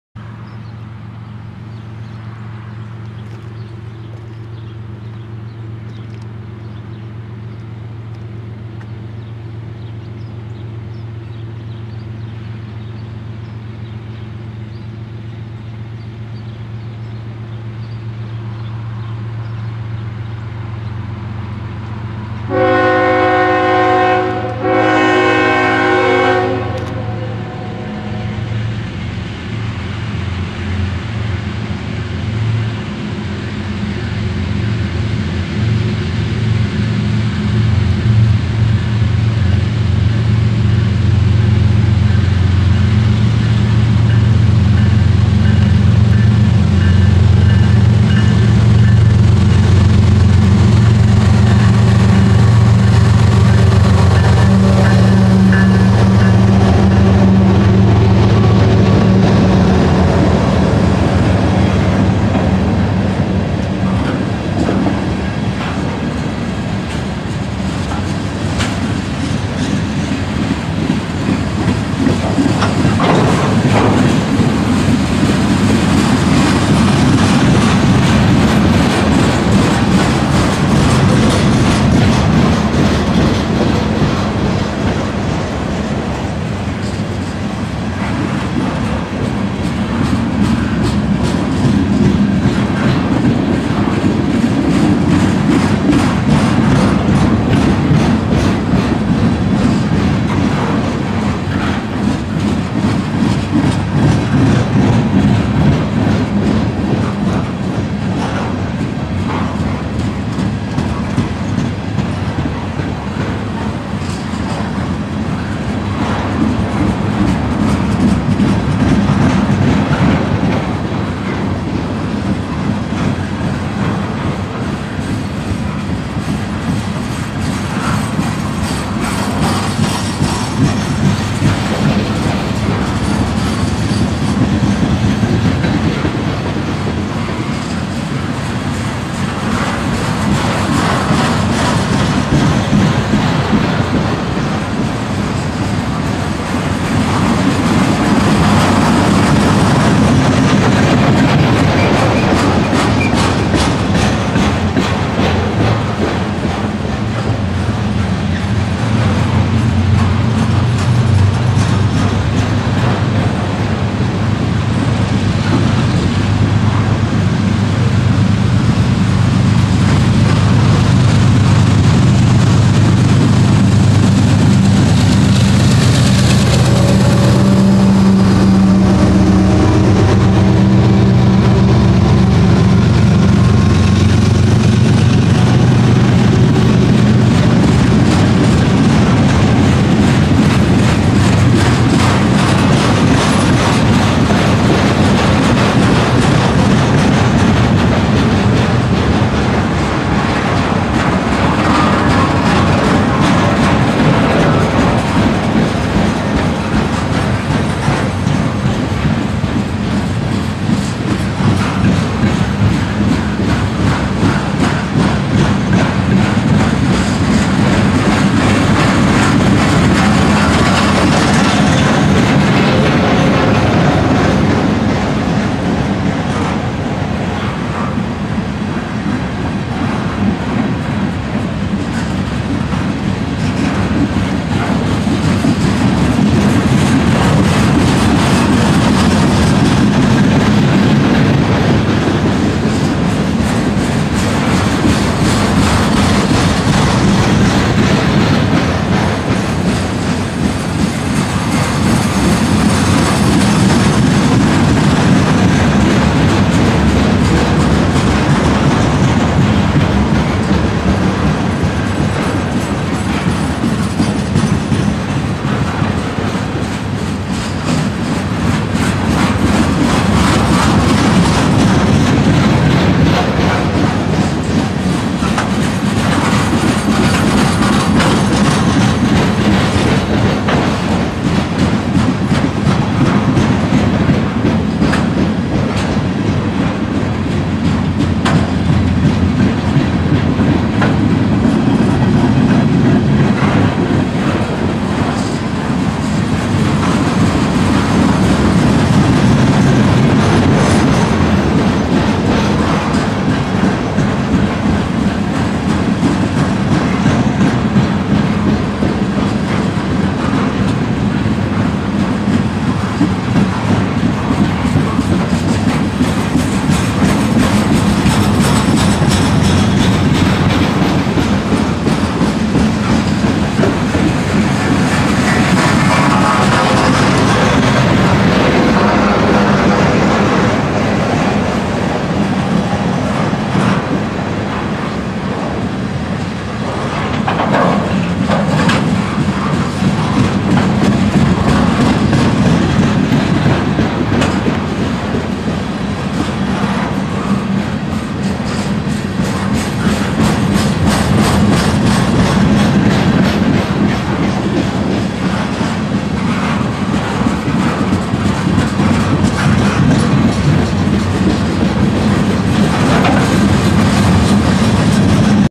[4K] Q301-31 is a All Trash Train south at St Denis MD
CSX Q301-31 a Mix freight Train is an All-Trash Train going southbound passing thru St Denis MD from Greenwich Yard - Philadelphia, PA to Rocky Mount, NC